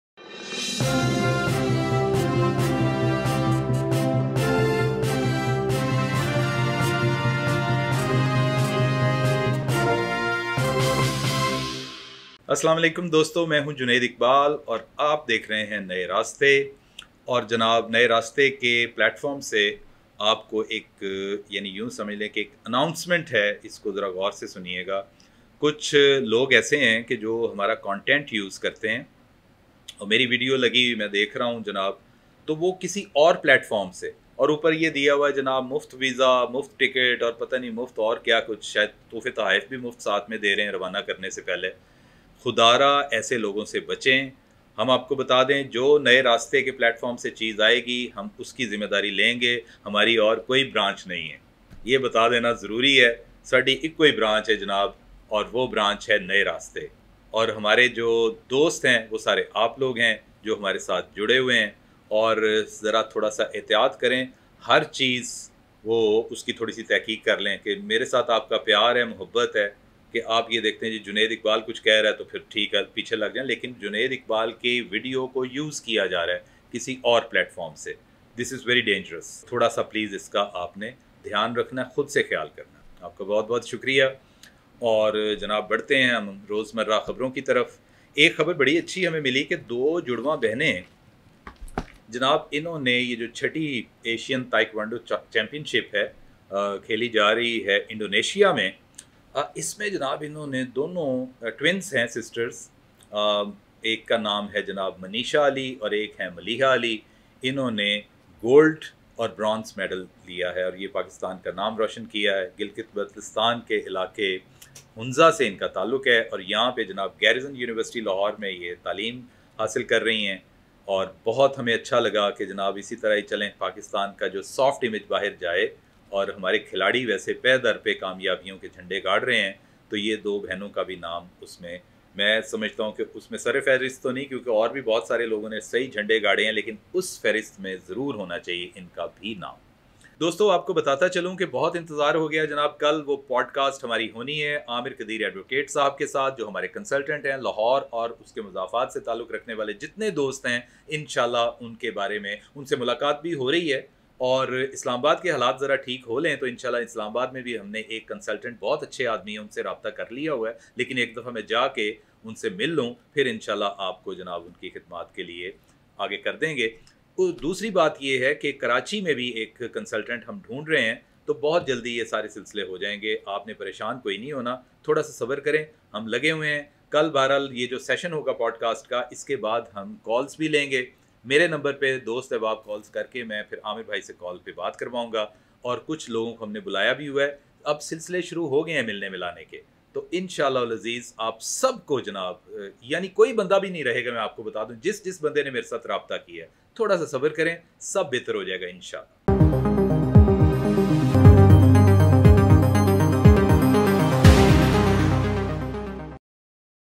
Special announcement for different cities of pakistan | Pakistani daughters making pakistan proud
Welcome to  channel Naye Rastey...!In this episode, Junaid Iqbal covers many important topics,fristly he is taking about Special announcement for different cities of pakistan ... he also discussed the  Pakistani daughters making pakistan proudDo share your feedback with us in comment section and stay connected with us